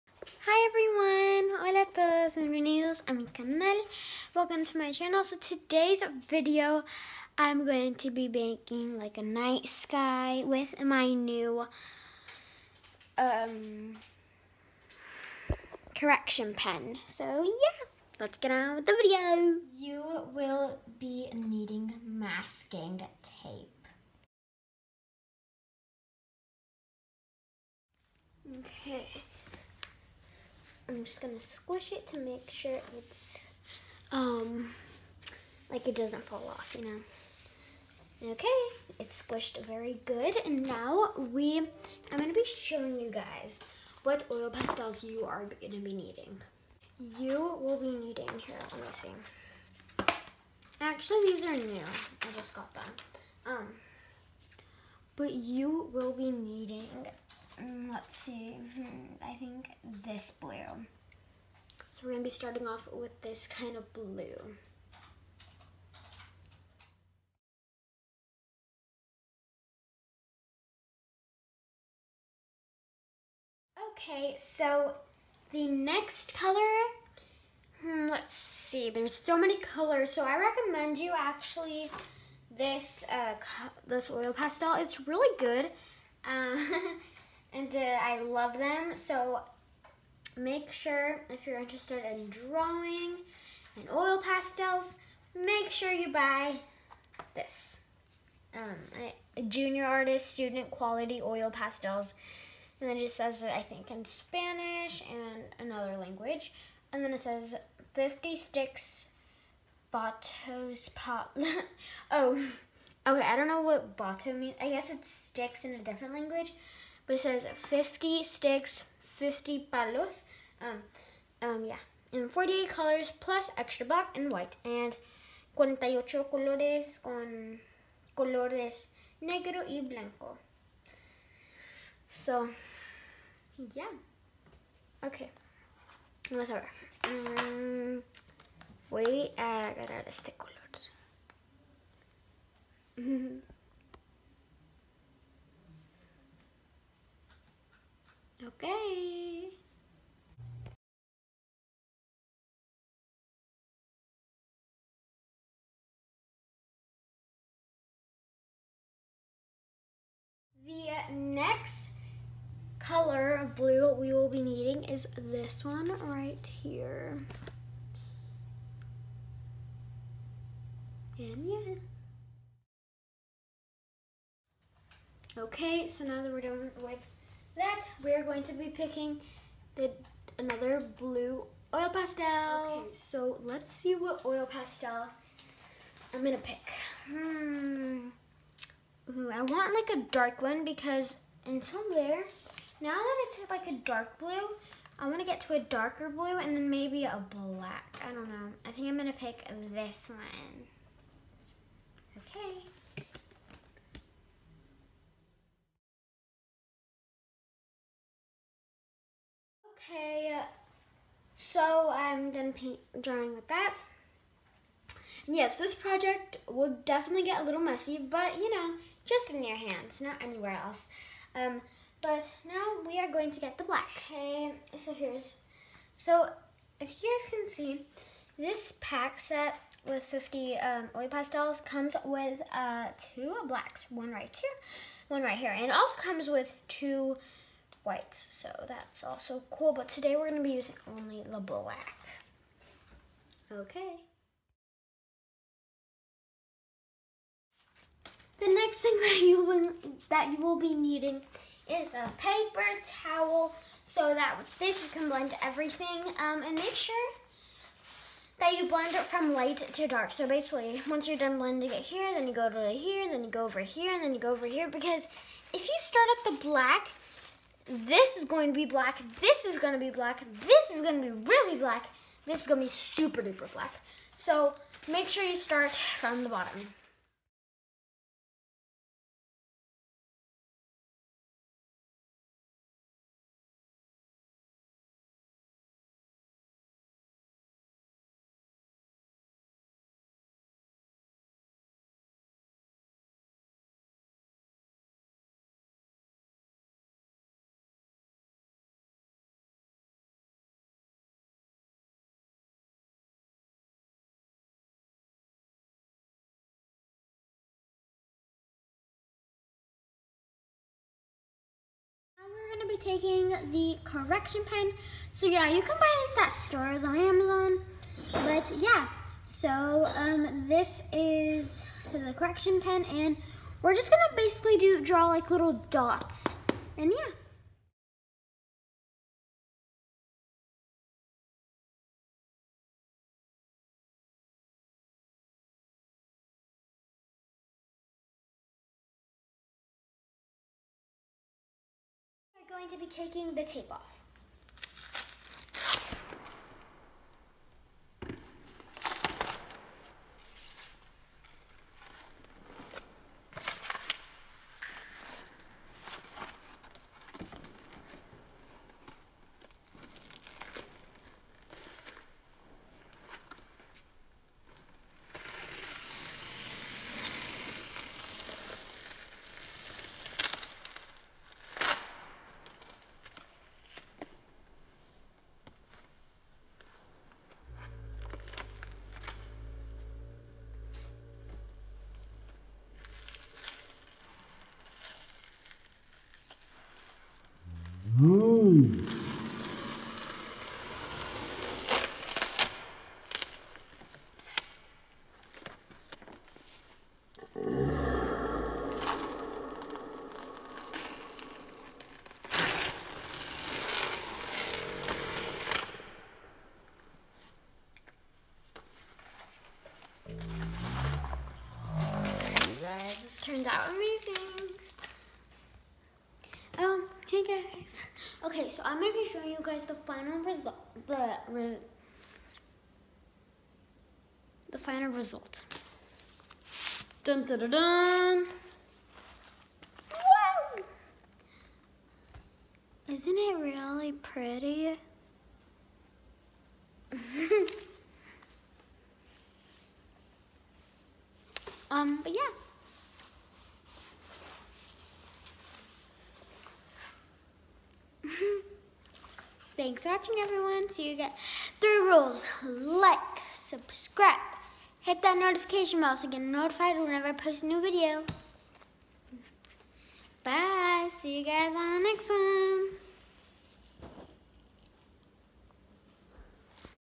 How to draw a Night sky 🌌 with stars ⭐️ using oil pastels and correction pen
Hi All

Today’s video is about how to draw a Night sky with oil pastel and a correction pen